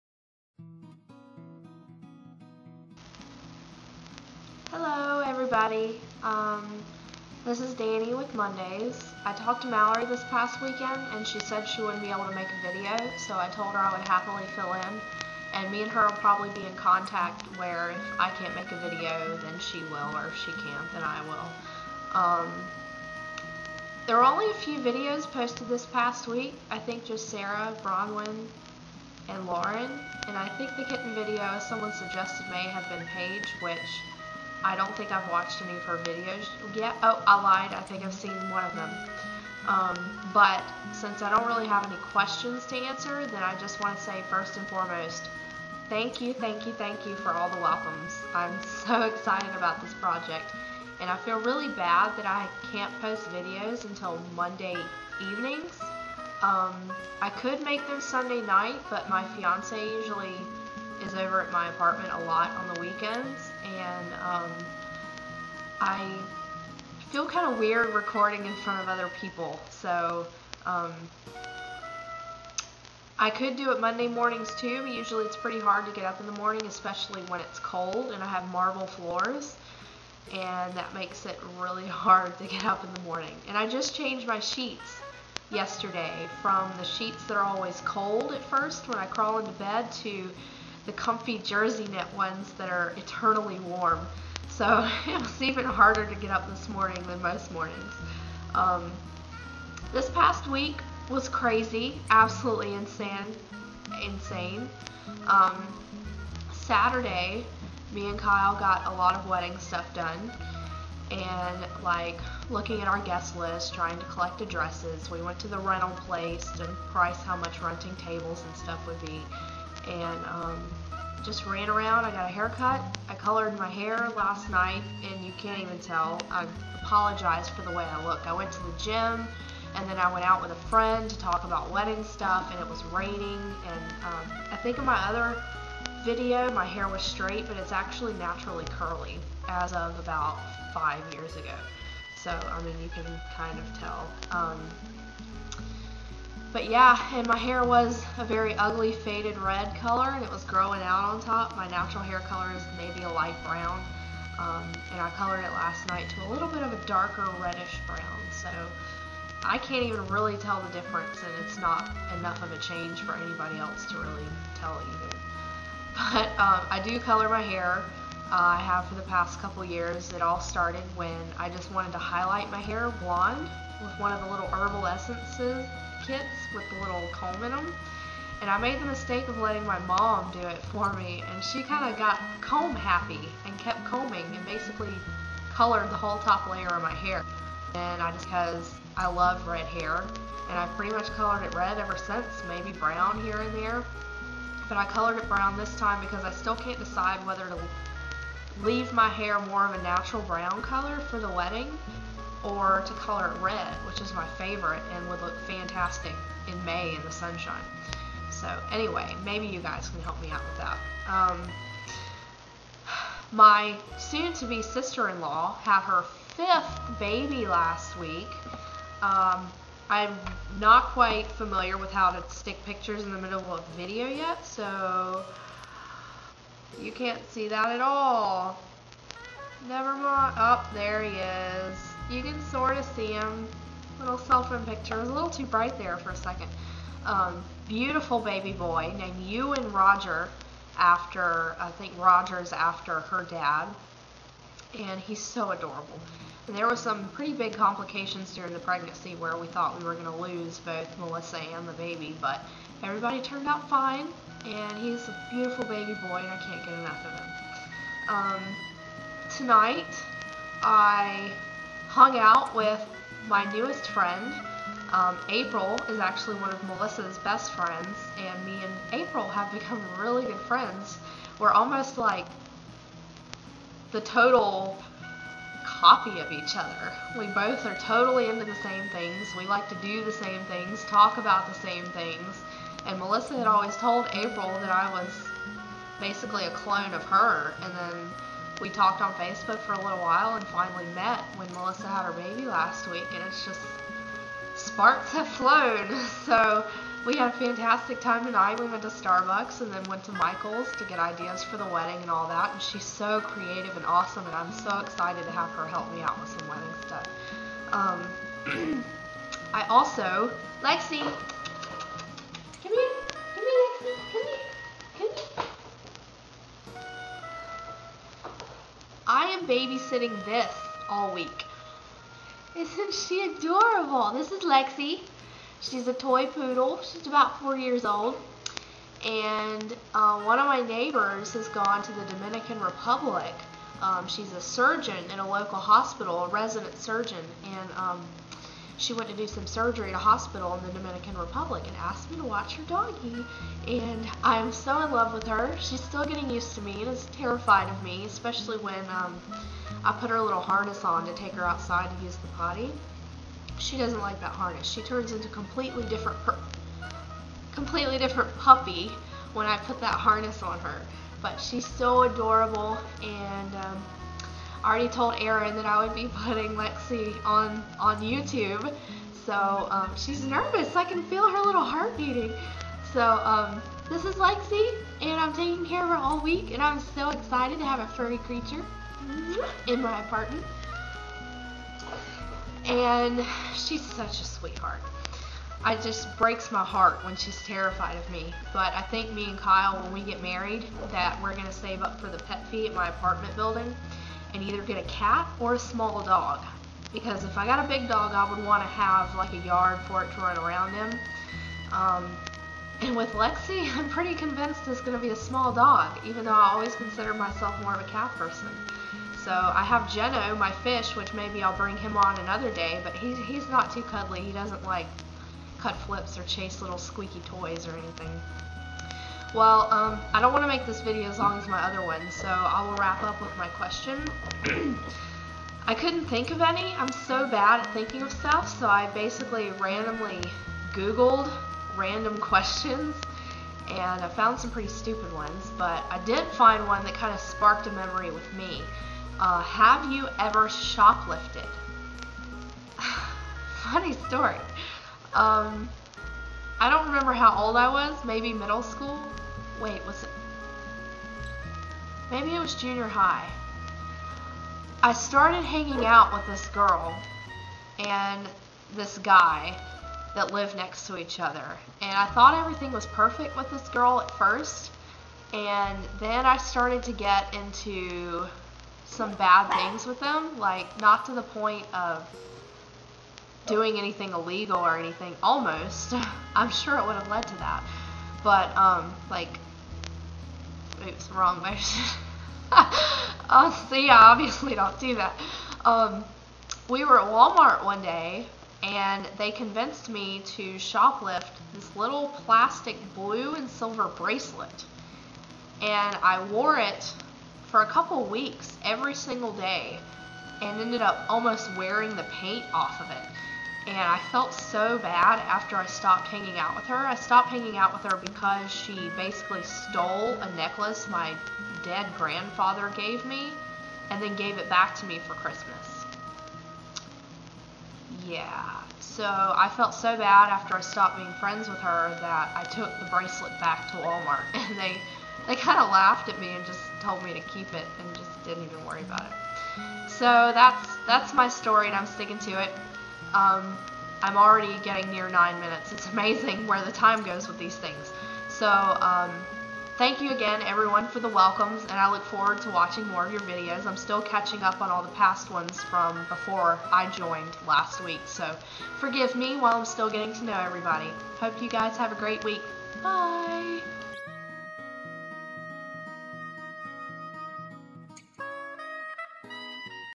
Latu [02/02] Busy Busy Busy Bee
Latu video for Monday, February 02, 2009. Description on that video follows:

"Threw this together at 10 pm...Had a long day, and just now had time to sit down for an hour to record and quickly edit!

The last week has been nuts...I'm puppysitting, I have a new nephew (well, he's be my nephew when I get married), and wedding plans are in full force. Bwaaaaaaaa

A millionth thank you for all the nice welcomes. =] "